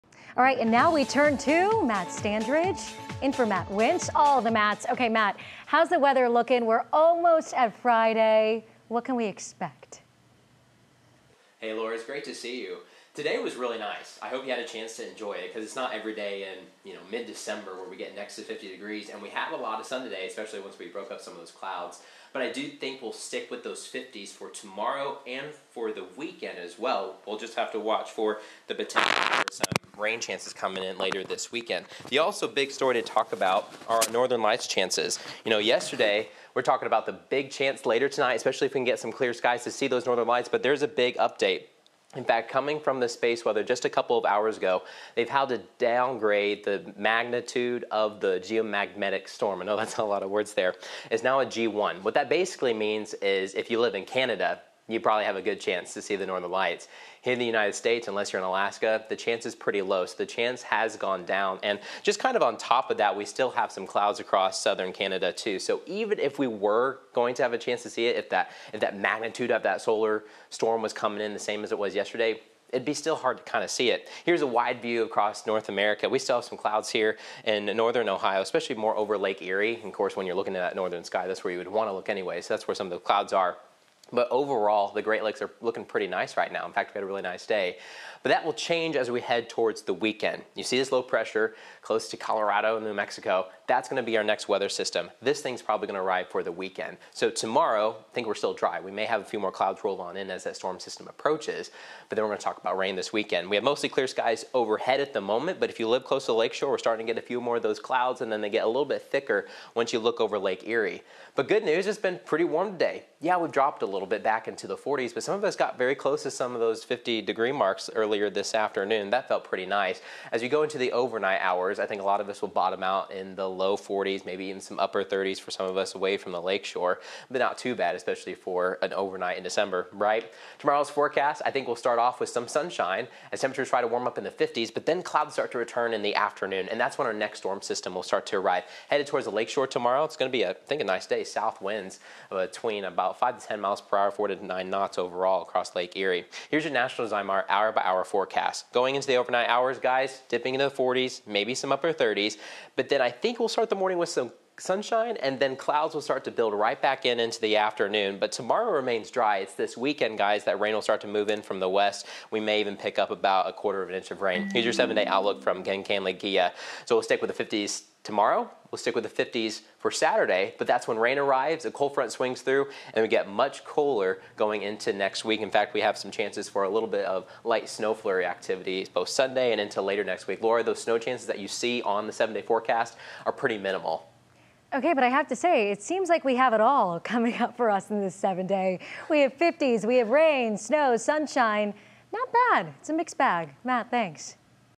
Sunny skies ahead of 50+ degrees in tonight's 5:00 p.m. weather forecast
Northeast Ohio is expected to warm into the low 50s both Friday and Saturday. However,  we're also tracking rainy conditions for the latter.